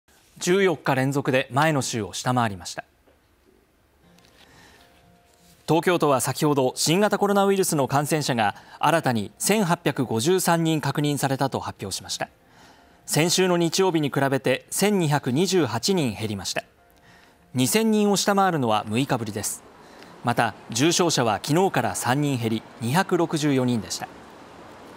東京1,853人感染 重症者264人（2021年9月5日）
14日連続で、前の週を下回りました。
東京都はさきほど、新型コロナウイルスの感染者が新たに1,853人確認されたと発表しました。
先週の日曜日に比べて1,228人減りました。
2,000人を下回るのは６日ぶりです。
また、重症者はきのうから３人減り、264人でした。